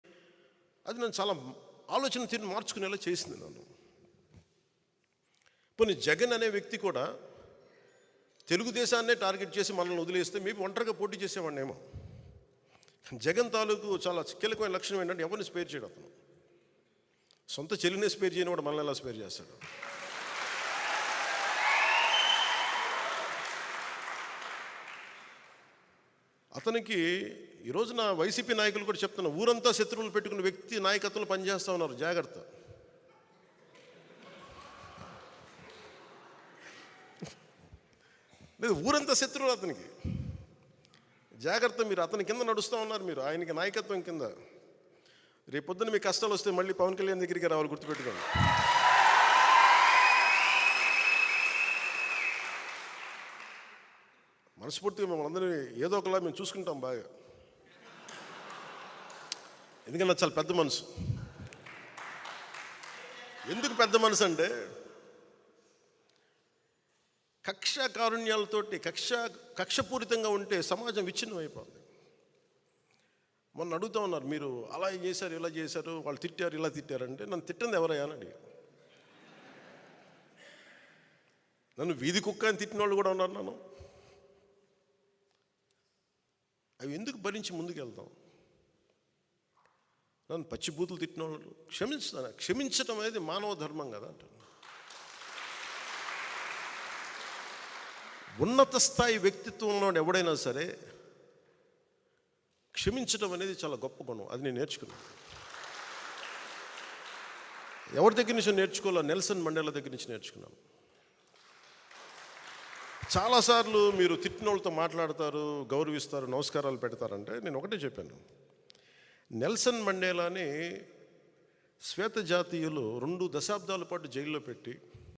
అయ్యో షర్మిల చెల్లి ? Pawan Kalyan Satires On Jagan Over YS Sharmila Issue | YOYO TV Channel
జగన్ చెల్లెల్నే లెక్క చేయలేదు Pawan Kalyan Satires On Jagan Over YS Sharmila Issue | YOYO TV Channel